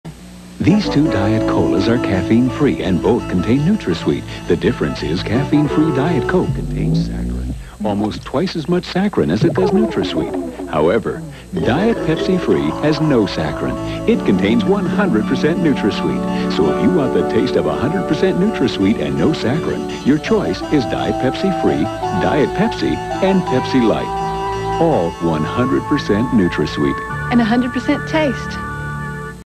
1985 Pepsi Free "100% Taste" TV Commercial
This commercial aired March 28th, 1985. It appeared during a broadcast of "Simon & Simon" on KGMP Channel 9.